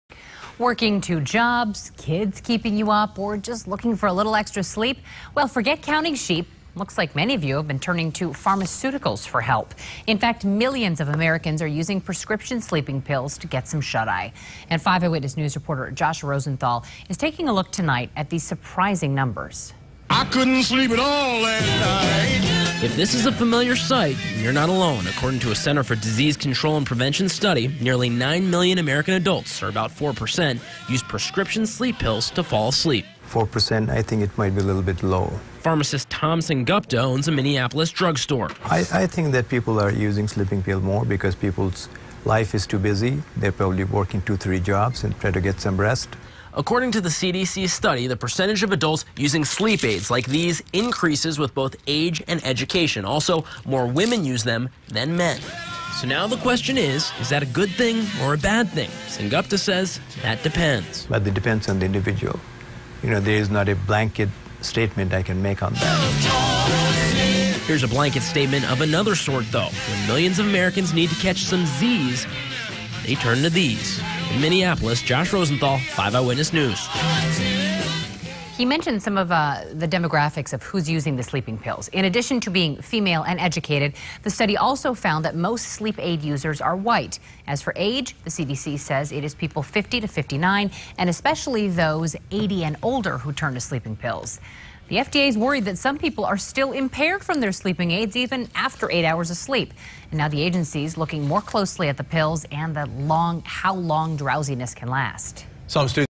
9 Million Use Sleeping Pills to Get More ZZZ's
A new study estimates that nearly 9 million adults in this country resort to sleeping pills to get sufficient rest. The Centers for Disease Control and Prevention says most are white, female, educated or 50 or older.